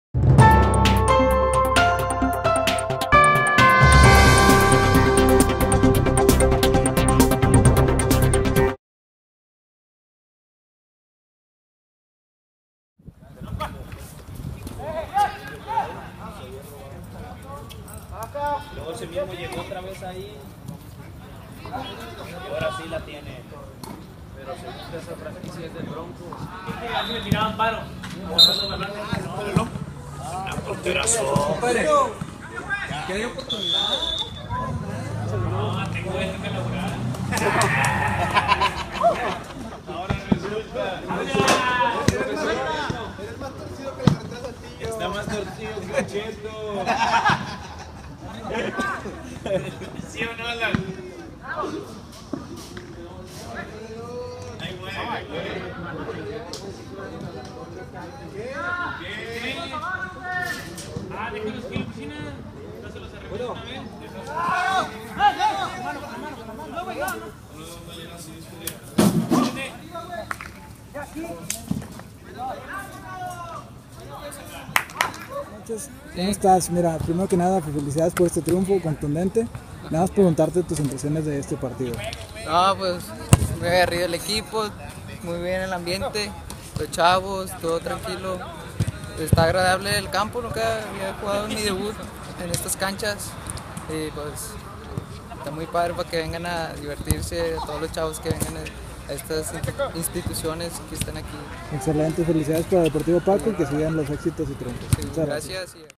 Intocables F7| Resumen 01 Noviembre 2016
El fútbol no se detiene y por esta razón les compartimos lo ocurrido en Intocables F7, recuerden , Mando #23 es el canal oficial de la liga, participen Intocables y no se pierdan de la jugada.